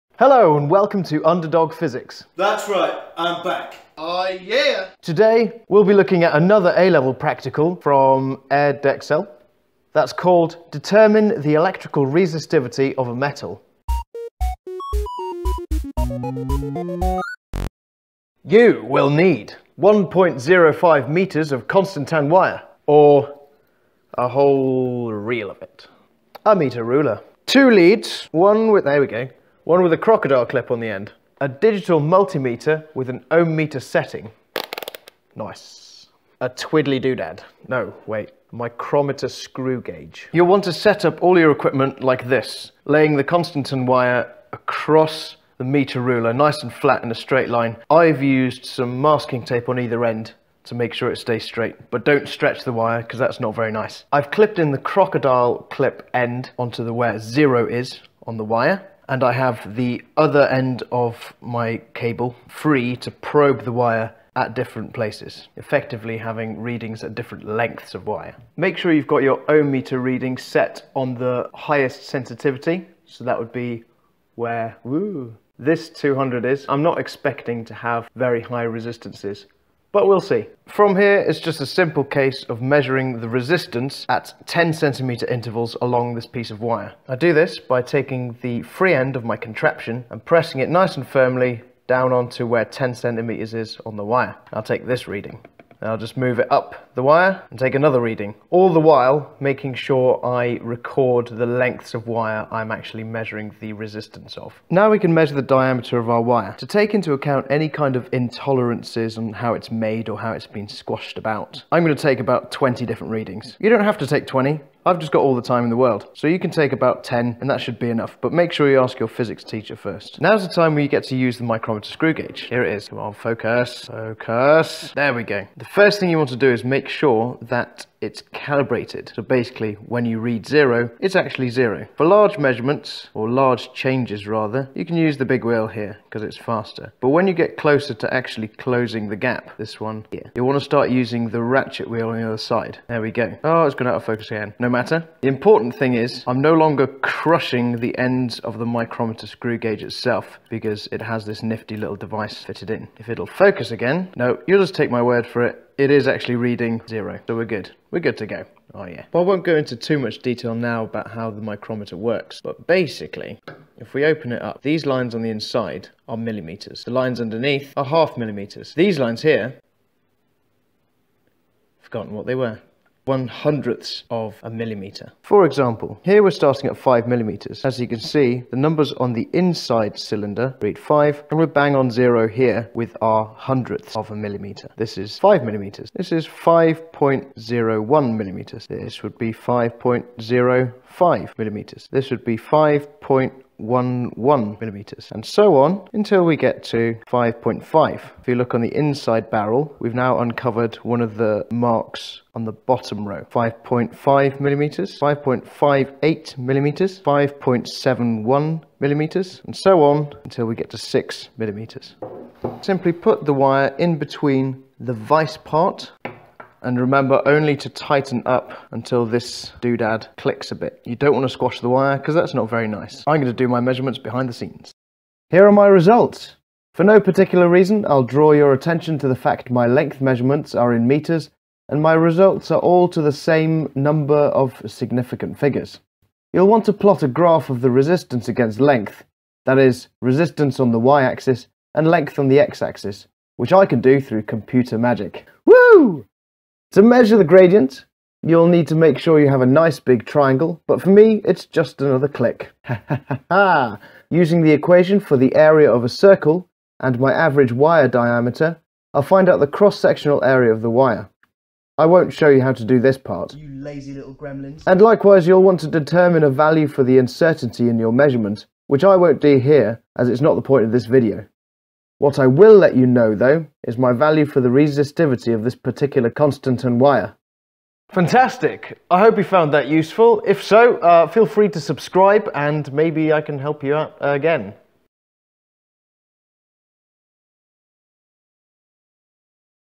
Resistivity of constantan wire - A Level Physics
A no-frills guide to the practical where you deduce the resistivity of constantan wire. I've even included a short introduction to the use of a micrometer screw gauge, which you may find useful anyway.